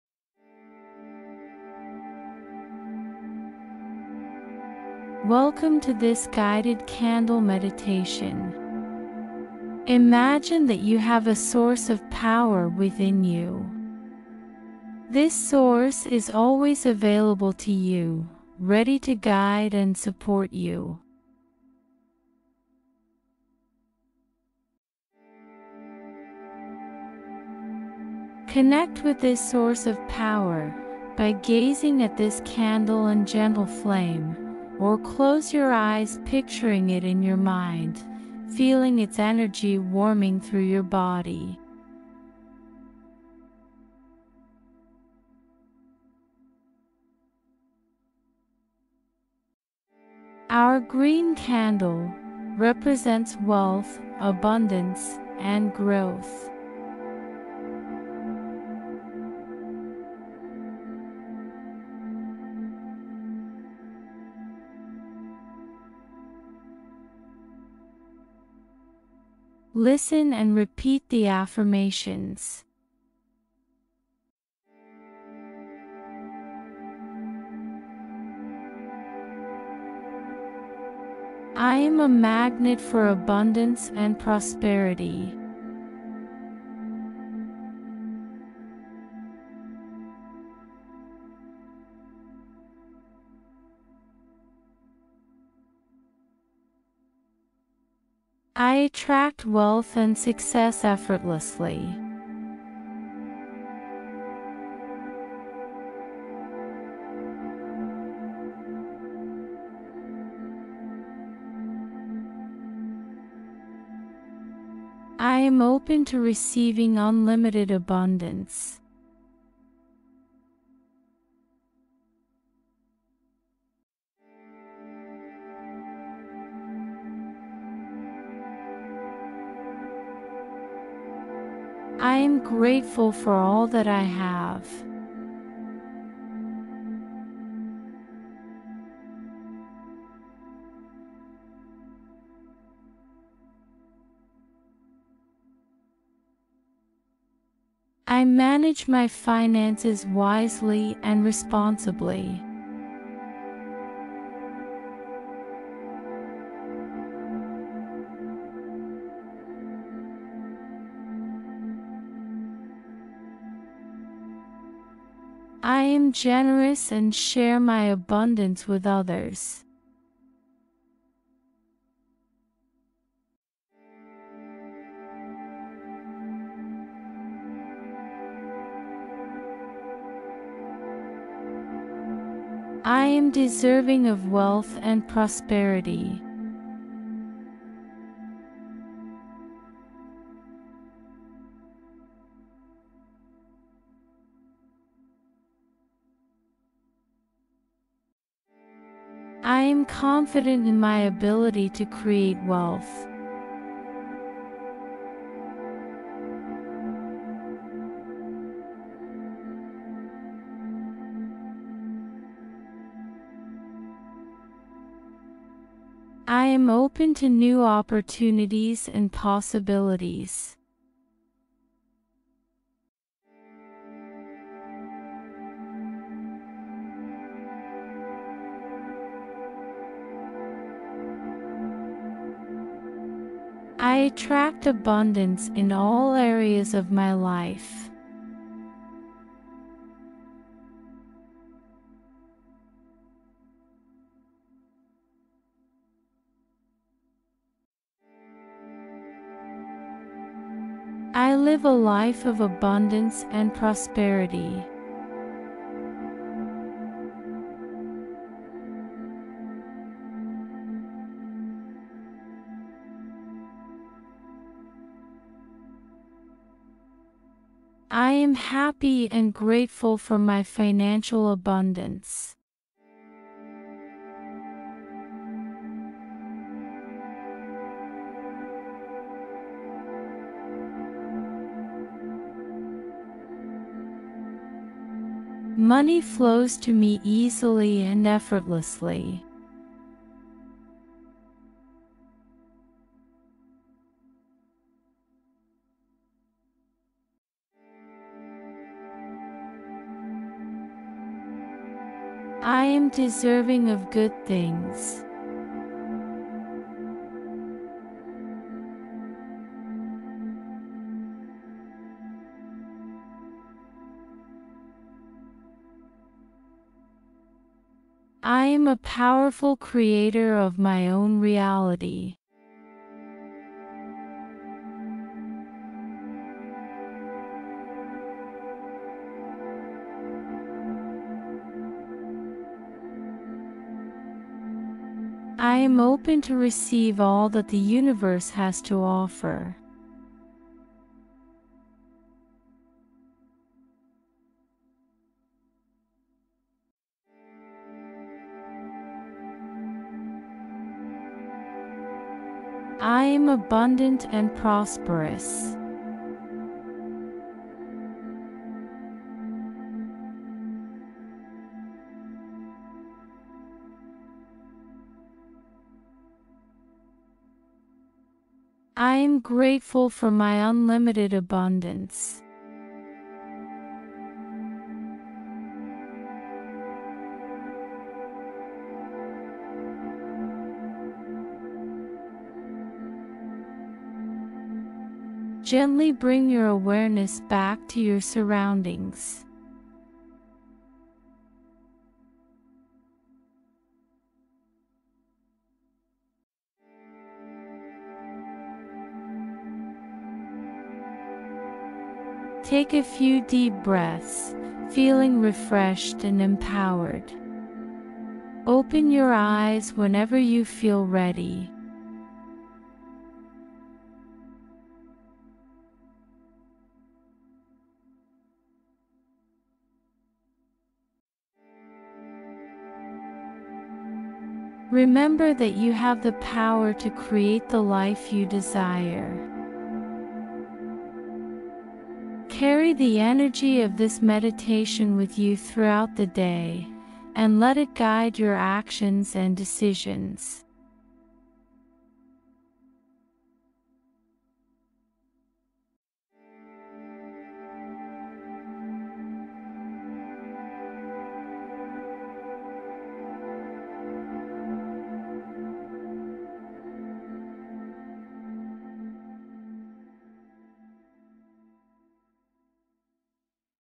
A Daily Meditation Universal Abundance and Prosperity Will Flow To You
Immerse yourself in a tranquil haven where progressive relaxation techniques and vivid visualisations intertwine, guiding you towards a state of profound serenity and profound self-compassion. A daily meditation to reinforce your positive intentions.